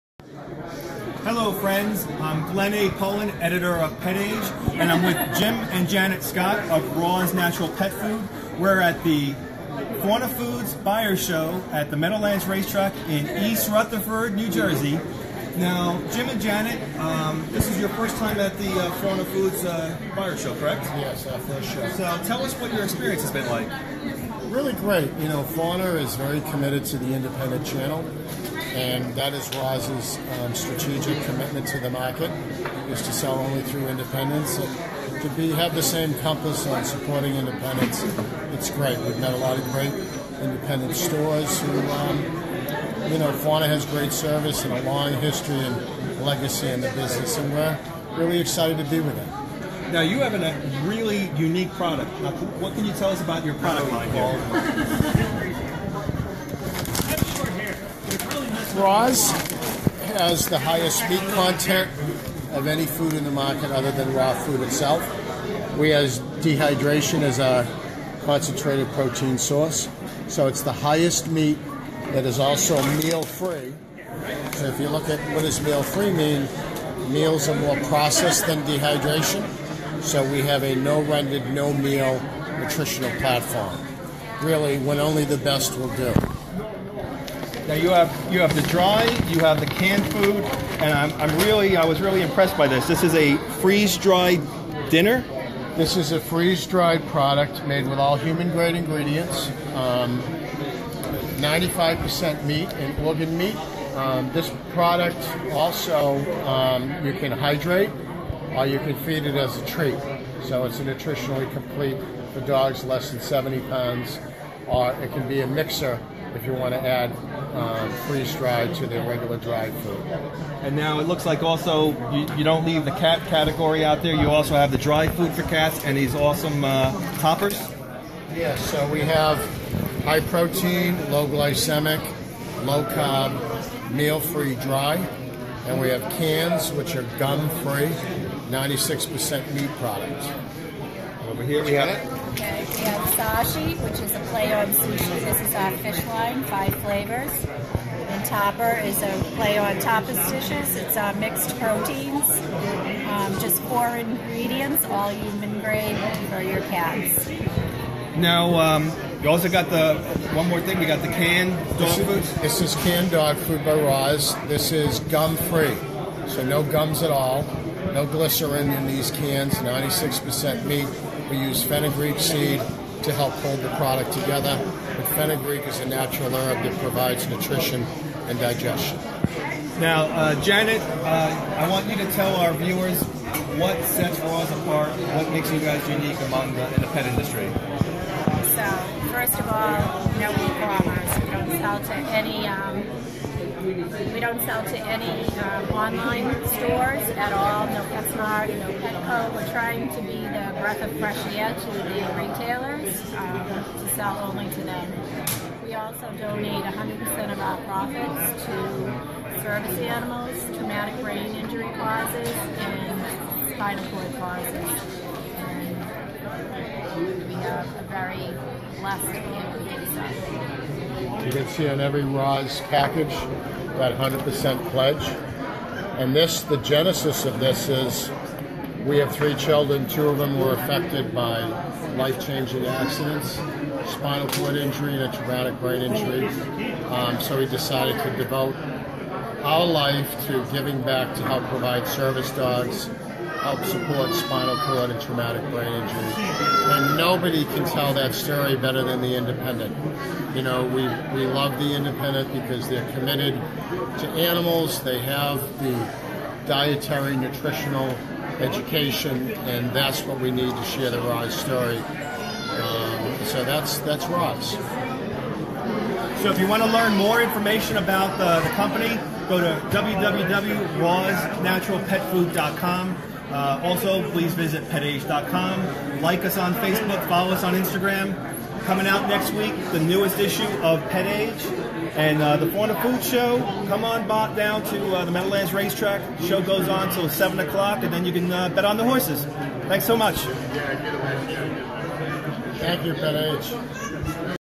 RAWZ Natural Pet Food
Pet Age speaks with Jim and Janet Scott of RAWZ Natural Pet Food from Maine at Fauna Foods Buying Show at Meadowlands Racetrack in New Jersey.